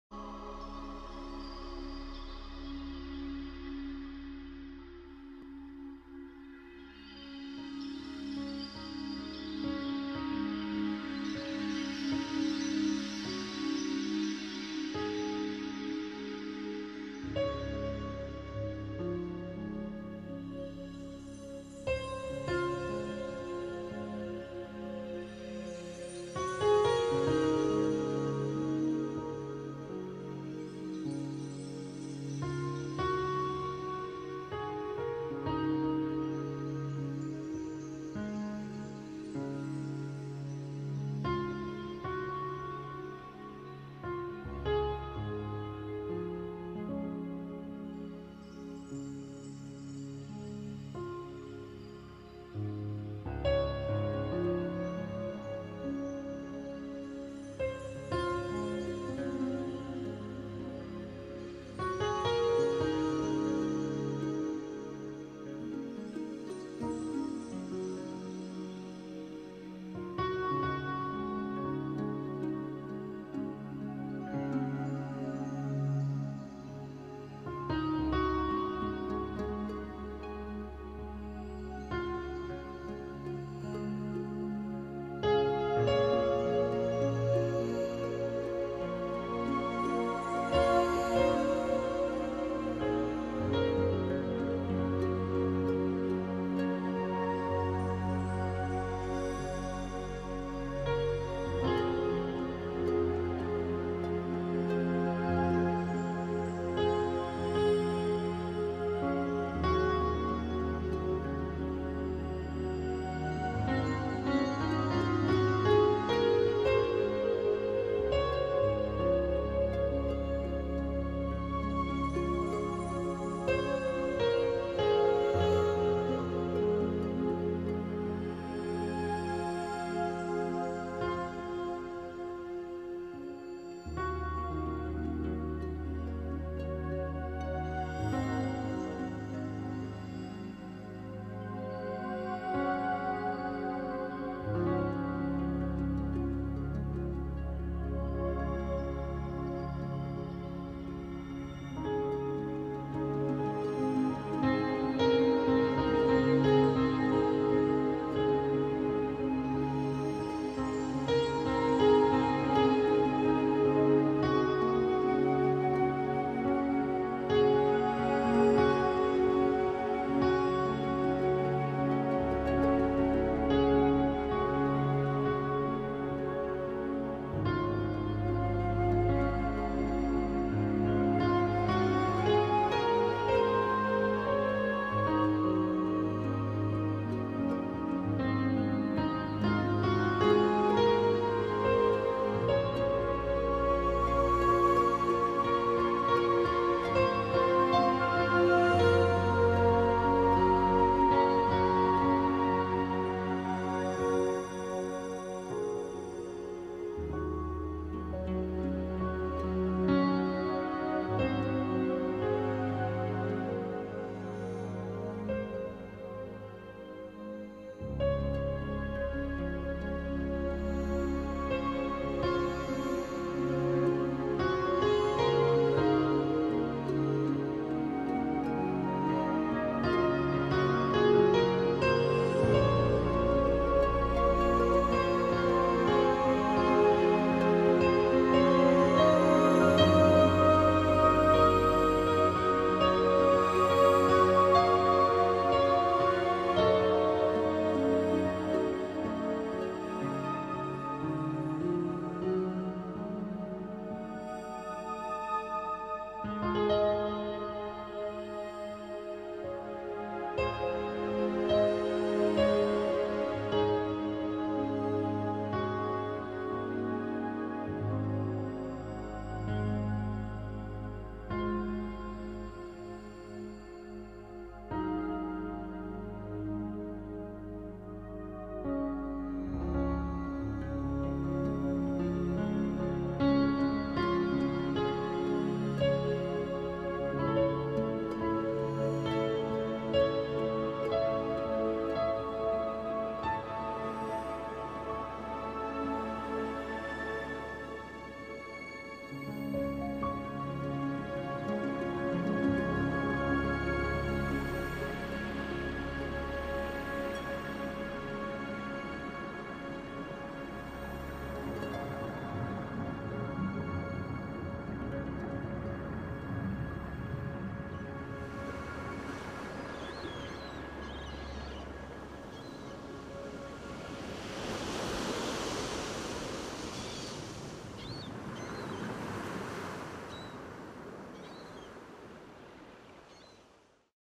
Funky de Bar Silver.wmv
Some nice clouds at sunset at Funky de Bar Hikkaduwa, Sri Lanka.